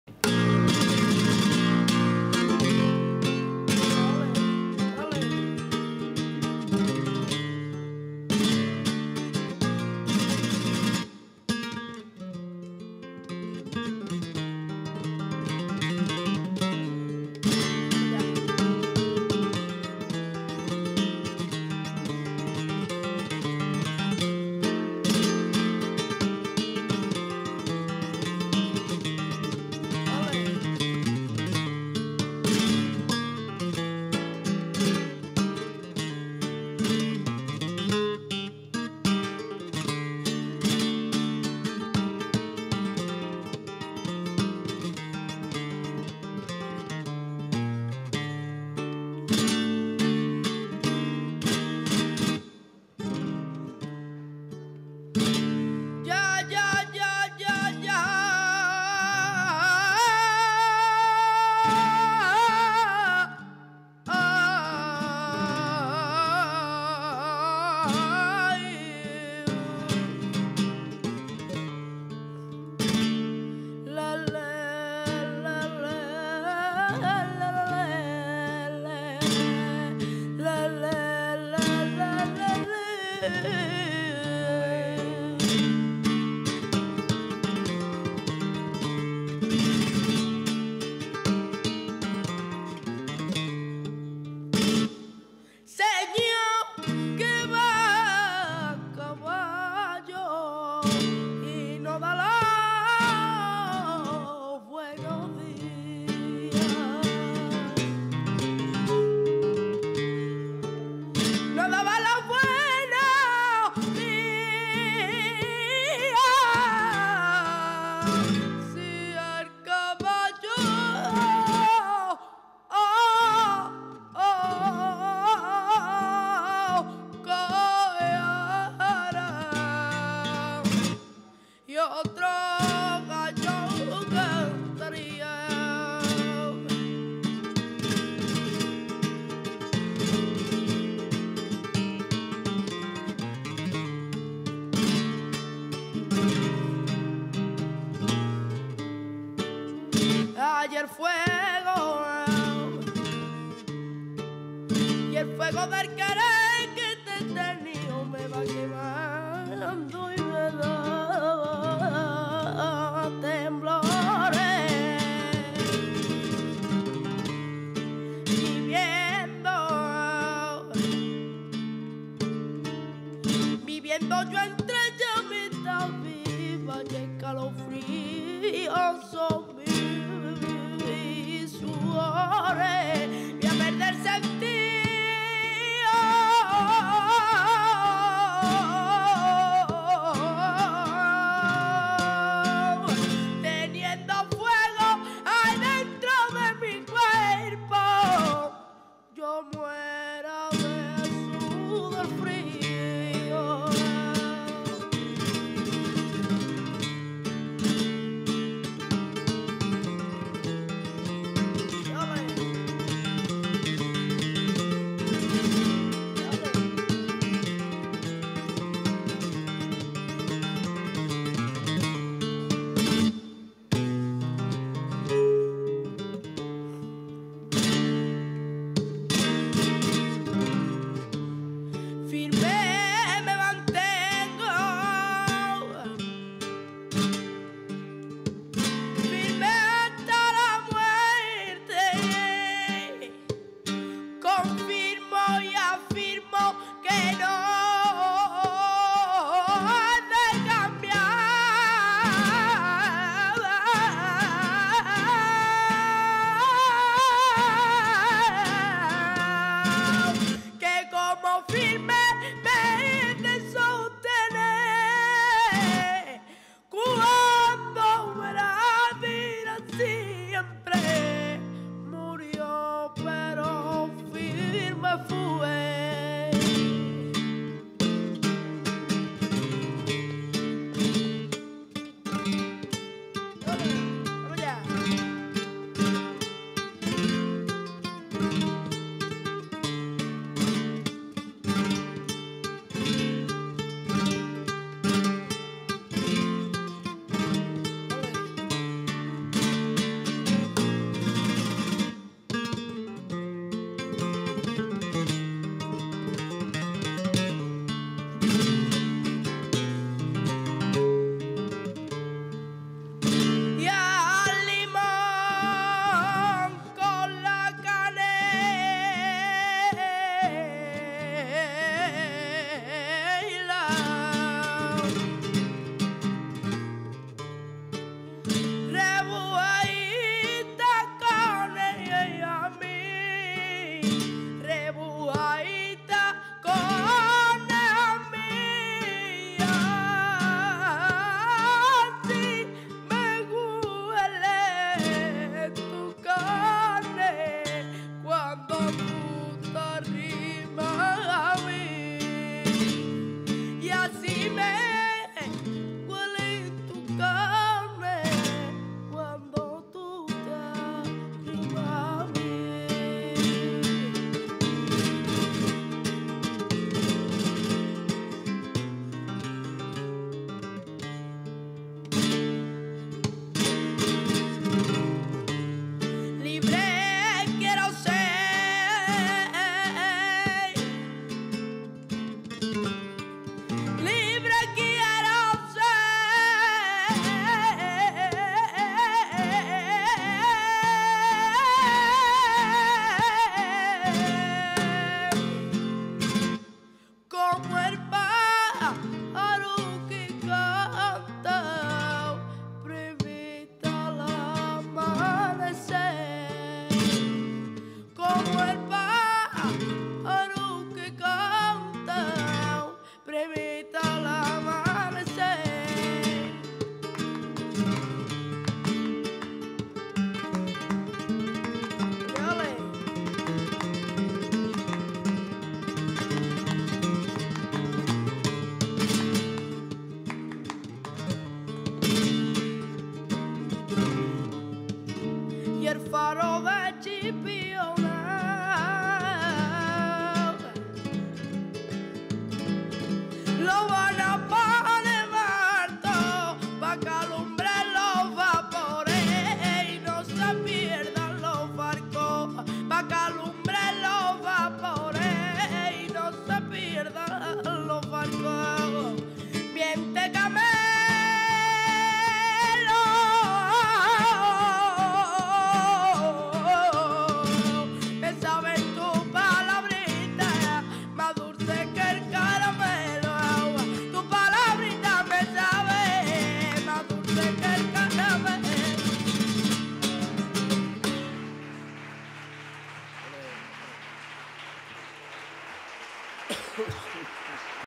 Luis Ortega y Manuel Peroles cante por TIENTOS XXXVI Concurso Nacional de Cante Flamenco Carmona
Luis Ortega y Manuel Fernández “Peroles” por TIENTOS XXXVI Concurso Nacional de Cante Flamenco “Ciudad de Carmona 2019”

Actuación del cantaor Luis Ortega López de La Algaba Sevilla,  acompañado a la guitarra por Manuel Fernández “Peroles”,  en  la fase de selección del XXXVI Concurso Nacional de Cante Flamenco "Ciudad de Carmona".
Cantando por tientos.

Performance by the singer  Luis Ortega López of La Algaba Sevilla. accompanied by Manuel Fernandez “Peroles”, in the selection phase of the XXXVI National Contest of Flamenco Singing "Ciudad de Carmona".
Singing for tientos.
Organized by: Flamenco Cultural Association "Friends of the Guitar".